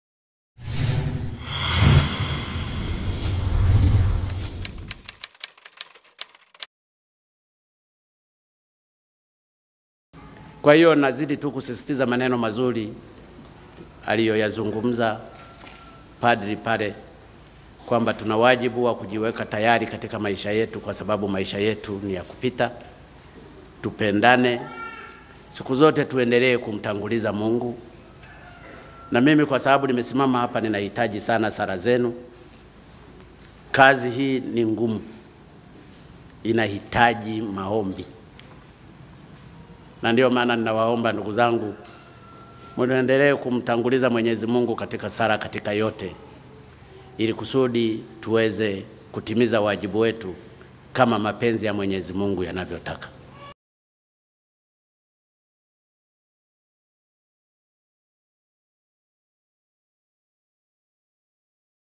Rais Magufuli Alichokisema Parokia ya Mlimani, Chato Mkoani Geita
Rais wa Jamhuri ya Muungano wa Tanzania, John Magufuli leo tarehe 25 Februari, 2018 ameungana na waumini wa Parokia ya Mlimani iliyopo Chato Mkoani Geita kusali Ibada ya Jumapili ya Dominika ya 2 ya Kwaresma.